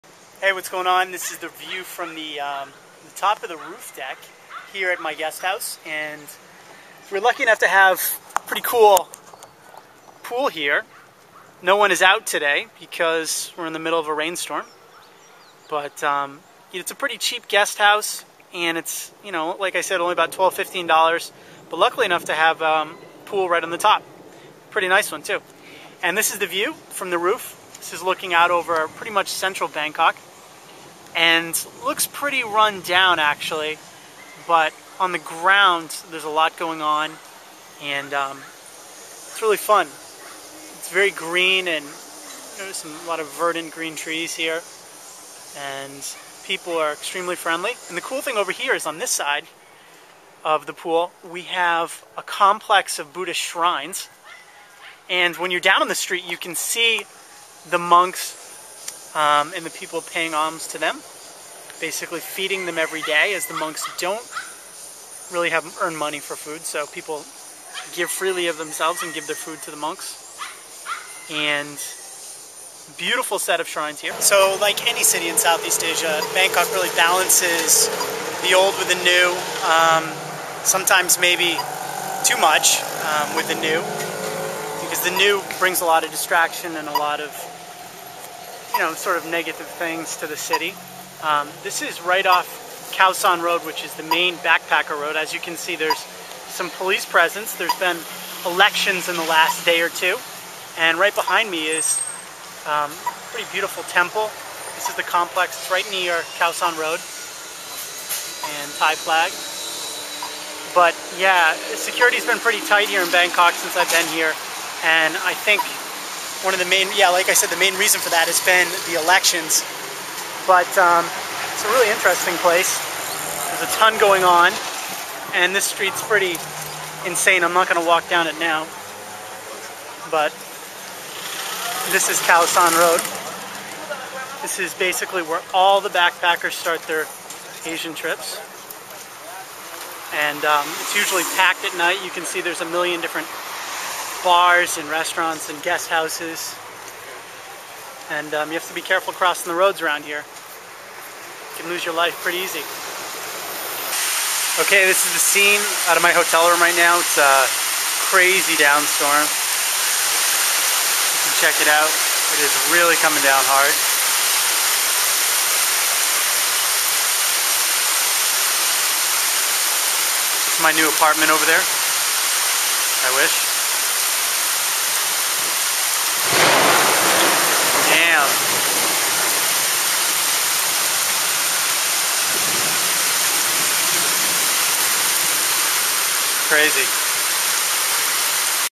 Bangkok, Thailand
Bangkok, Thailand - A short video recently shot on site in the Thai capital.

Recent unrest has plagued the city as protesters have demonstrated against the current government which they see as linked to the former elected Prime Minister Thaksin Shinawatra, who now lives in exile in Britain.

Dan Snow is an internet entrepreneur and has spent the last two months living and working in Asia.